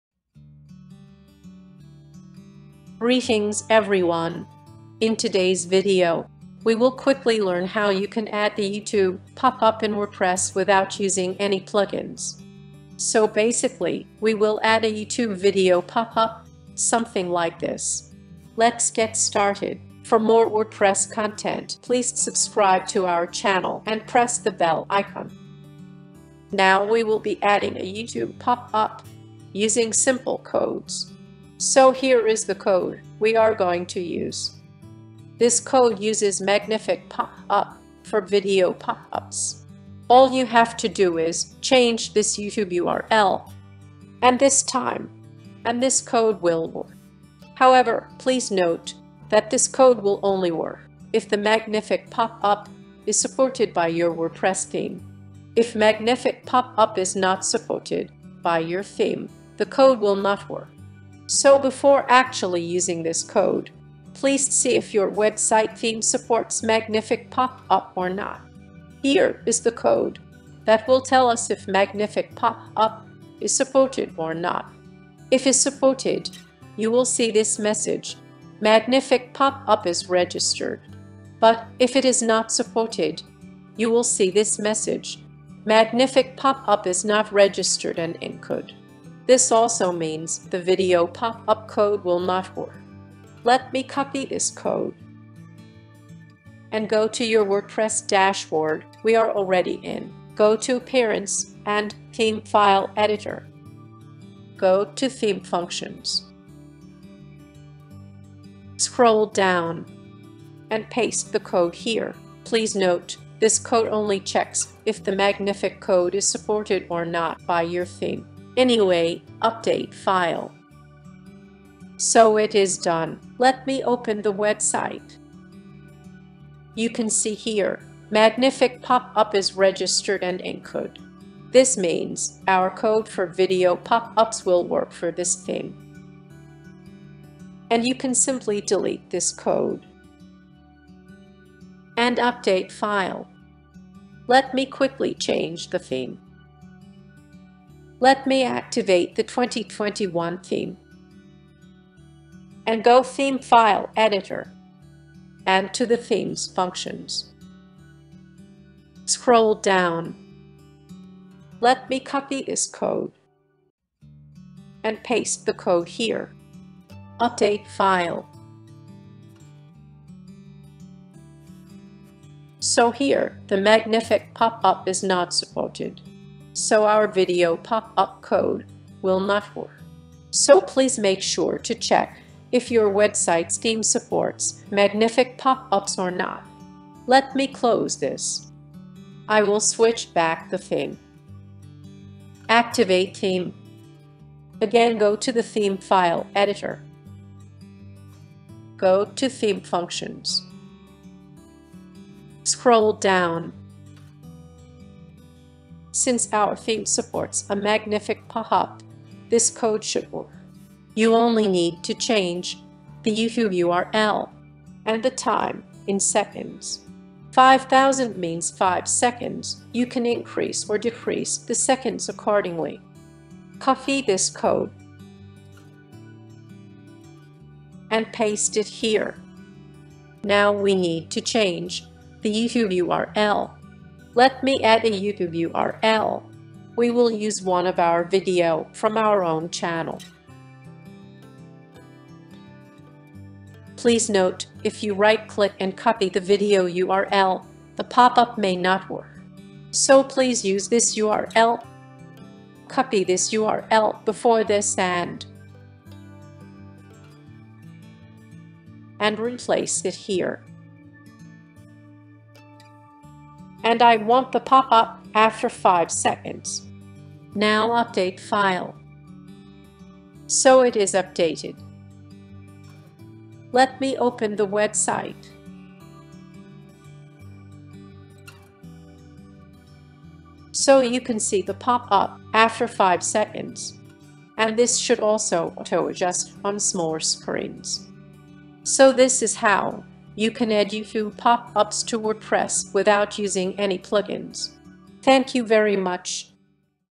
How to add a YouTube video popup in WordPress without using any plugins? Wordpress 66
In this WordPress tutorial, we will learn how to add a YouTube pop-up in WordPress without using any plugins. We will use a simple code that uses magnific pop-ups for video pop-ups. Adding a pop-up to your WordPress website is a great way to grab your audience's attention and increase engagement.

Please note that magnific pop-ups may not be supported by all WordPress themes. Thus, it is highly recommended that you check if your theme supports a magnific pop-up - you can use the provided code to do so. After confirming that your theme supports magnific pop-ups, you can copy and paste the YouTube Video popup code to your website's theme functions. You will only need to change the YouTube URL and the time after the popup appears. The time in the code is in seconds, so please customize it as per your requirements.

With this tutorial, you can easily add a video pop-up to your WordPress website without using any plugins. Please give it a try and let us know if the code worked for you or not. Thank you very much.